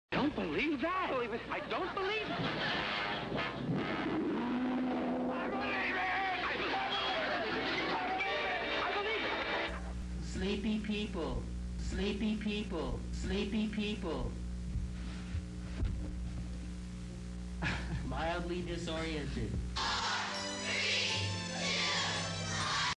Sleepy People - Promo for Cable Access Telethon
I think this is from 1983 and features the Minneapolis band, Sleepy People.
Promo for the yearly Cable Access Telethon in Fridley, Minnesota.